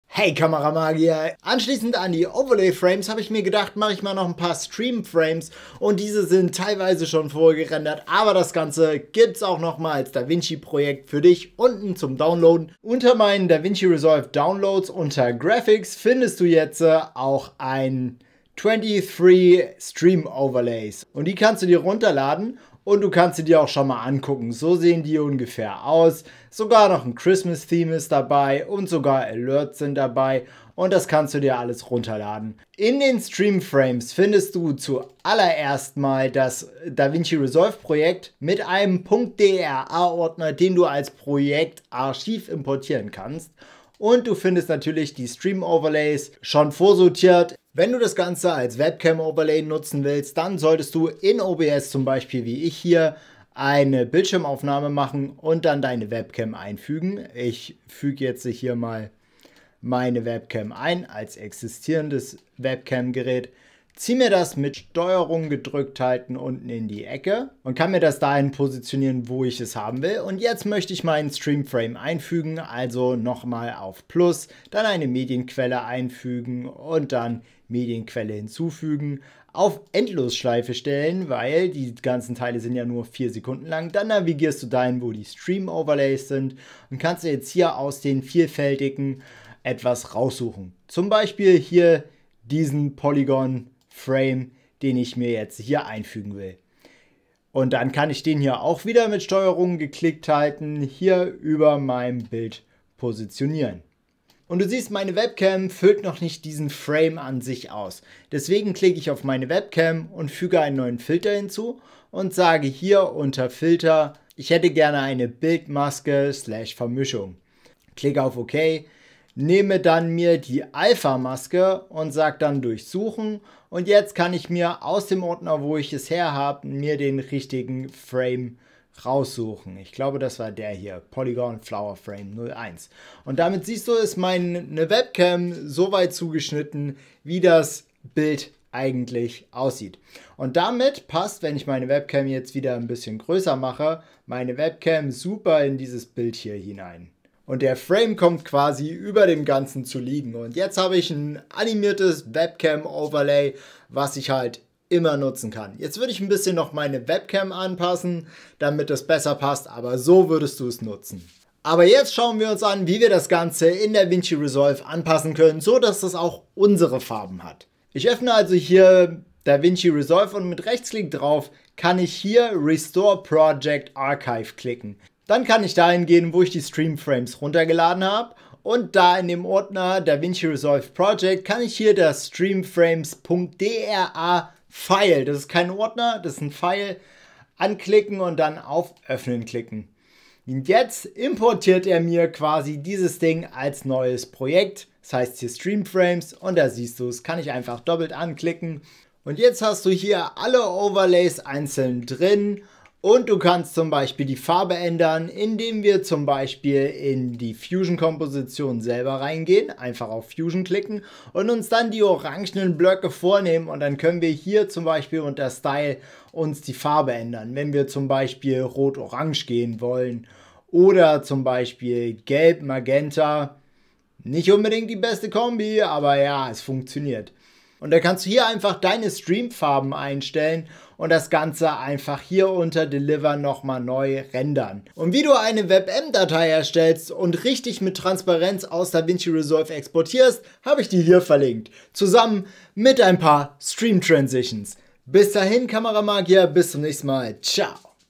Free Stream Overlays + Davinci Resolve Projekt File
↓Timestamps↓ ­­­­­­­­
0:00 Free Stream Overlays
0:12 Download
0:47 Streamframes in OBS anwenden
2:41 Farben ändern & Davinci Resolve Projekt importieren
2:47 Projekt Archiv improtieren
3:19 Farben ändern
3:54 Als WebM Rendern